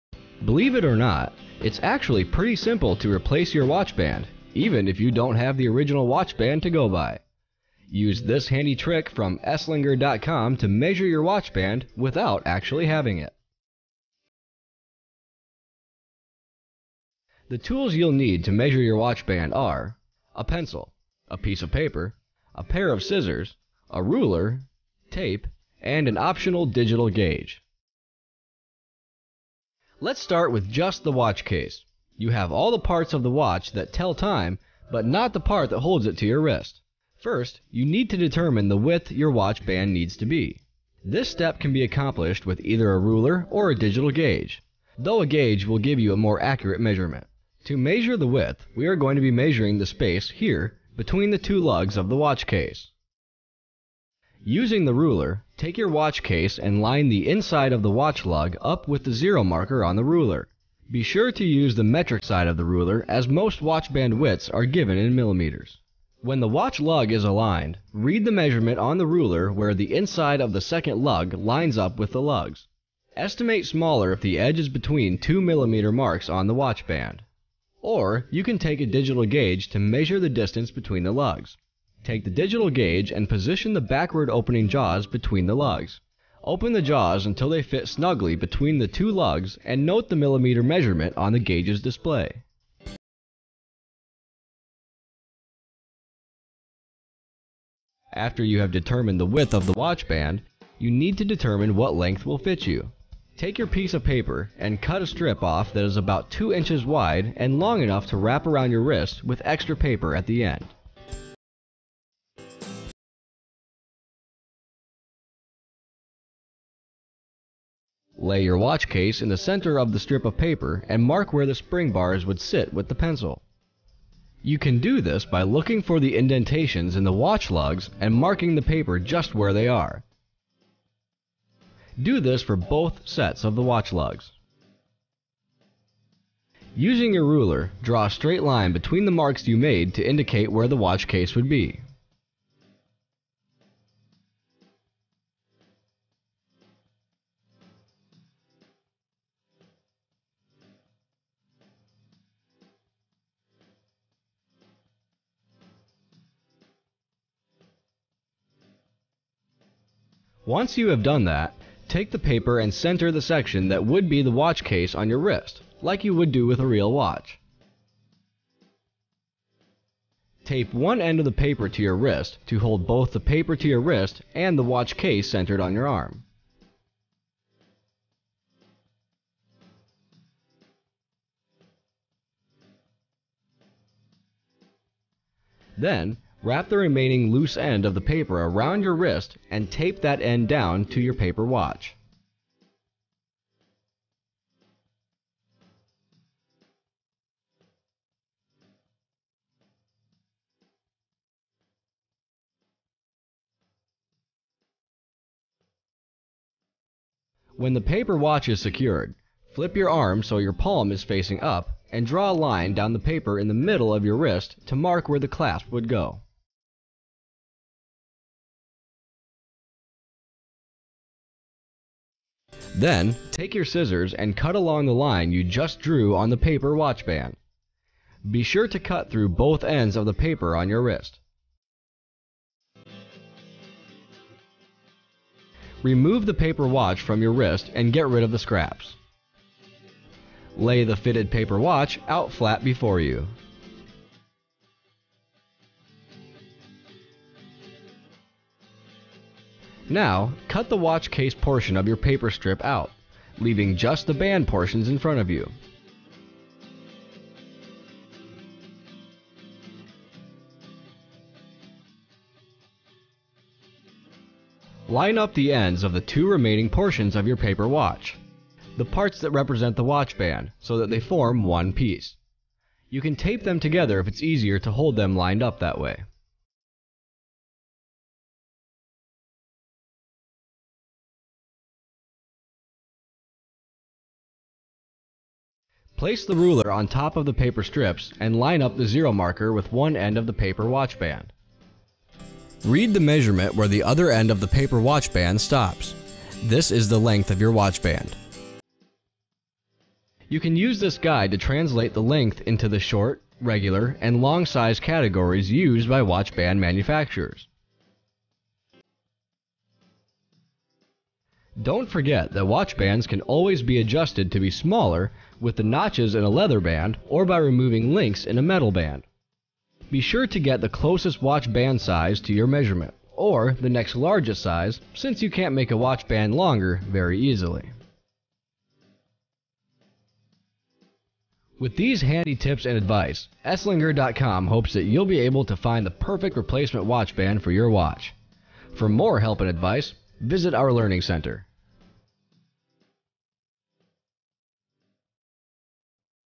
DIY How to Measure for a Watch Band without a Watch Band
No tools or experience required - simple step by step instructions on DIY watch band replacement.